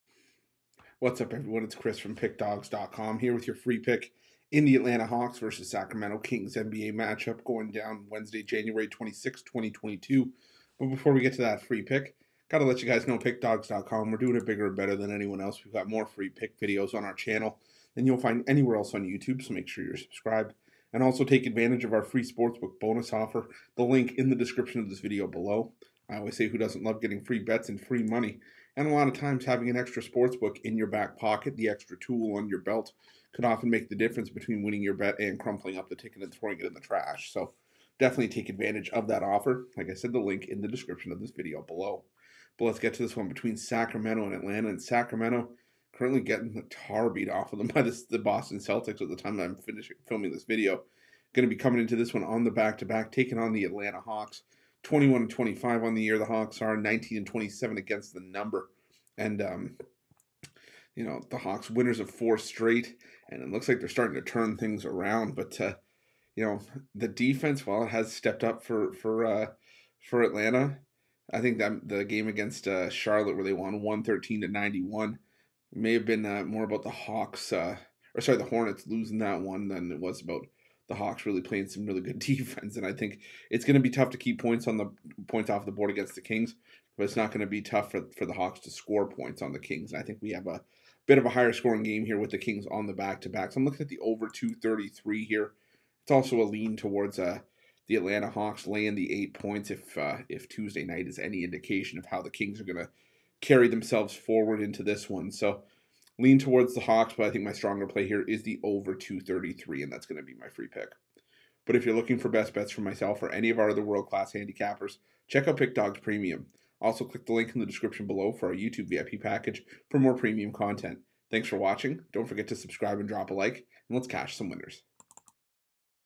Atlanta Hawks vs Sacramento Kings 1/26/22 Free NBA Pick and Prediction NBA Betting Tips
The Sacramento Kings face the Atlanta Hawks in Wednesday's NBA Basketball action. Get Mitch's NBA Pick and Prediction for this game.